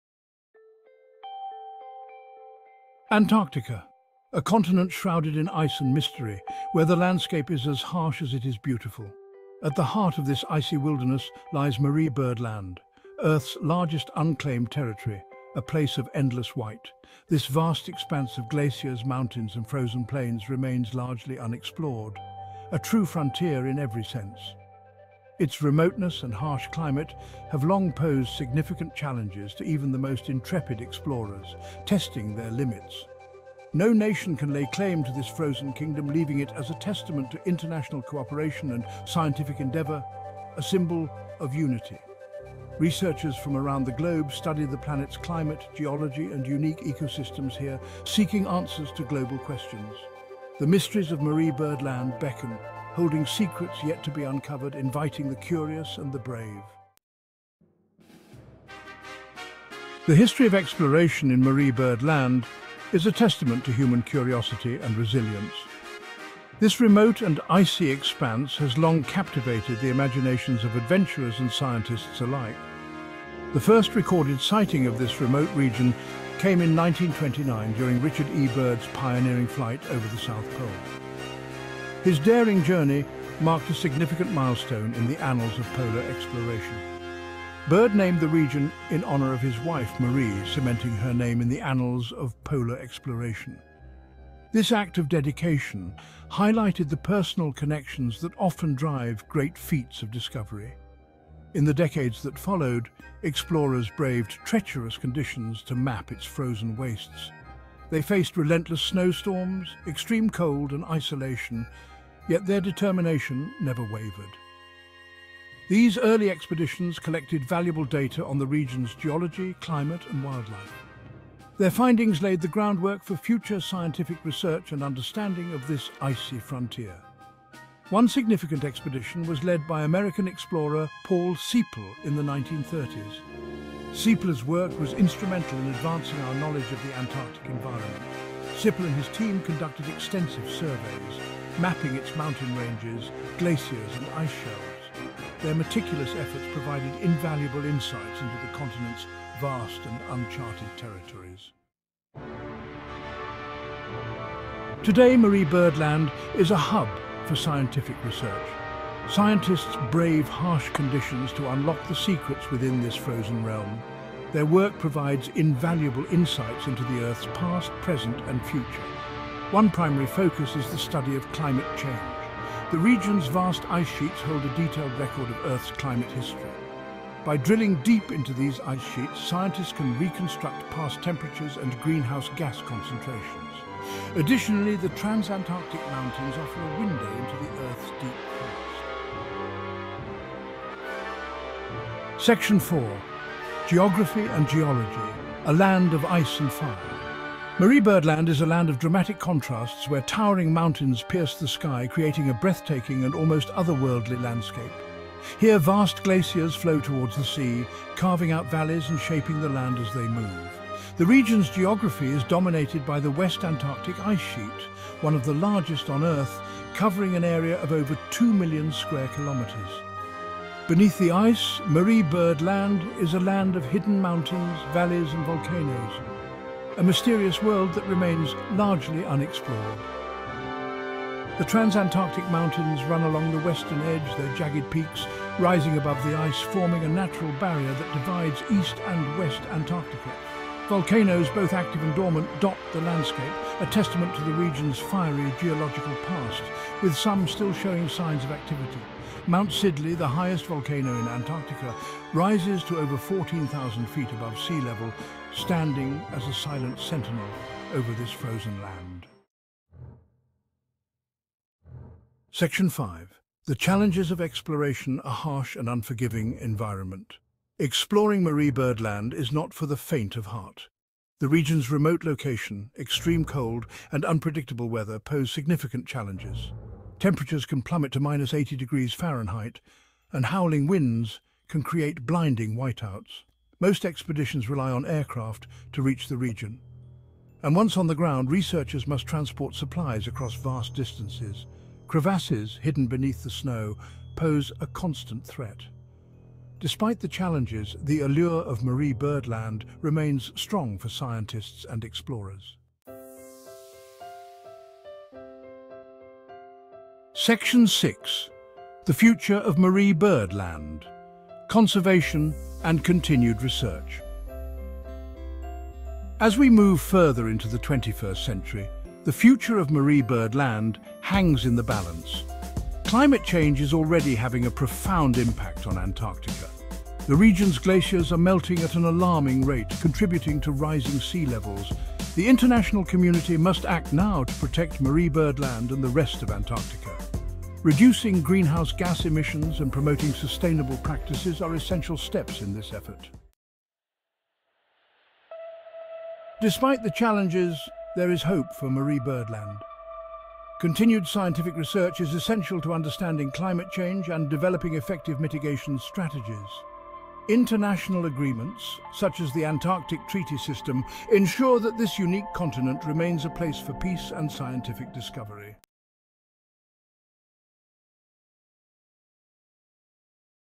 The Earth's Largest Unclaimed Territory
Discover the Mysteries of Marie Byrd Land: Earth's Largest Unclaimed Territory 🌍❄️

Join us as we delve into Marie Byrd Land, a remote and unclaimed expanse of Antarctica. Learn about its first sighting in 1929 by Richard E. Byrd, and how it was named after his wife, Marie. Explore the harsh conditions that challenge scientists in their quest to uncover the region's secrets. From the formidable West Antarctic Ice Sheet to hidden mountains and volcanoes, discover how researchers study climate change and geology amidst extreme cold and dangerous terrains. Highlighting the vital role of international cooperation and urgent climate action, this video is a journey into one of the last great frontiers on Earth.